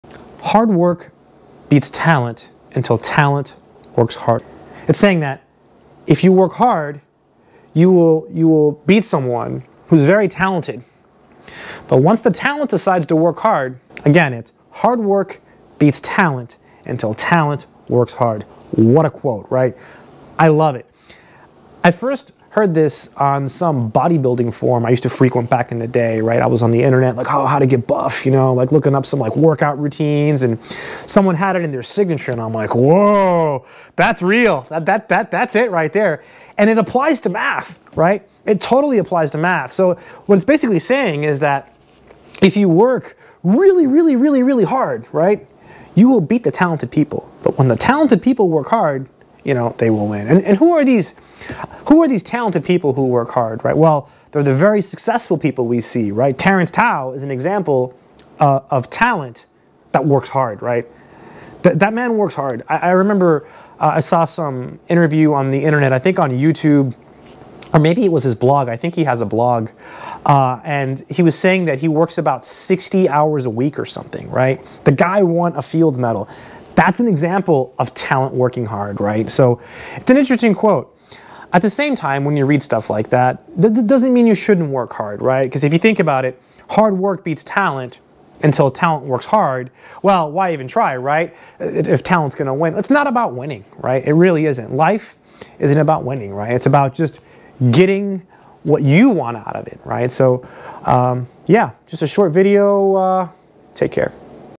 Hard Work Beats Talent Until Talent Works Hard
I once read that "Hard Work Beats Talent Until Talent Works Hard". This is an interesting quote and in this video I talk about it and discuss how it relates to mathematics. There are plenty of examples of talent that works hard, for example Terence Tao. In the end, does it matter? What do you all think?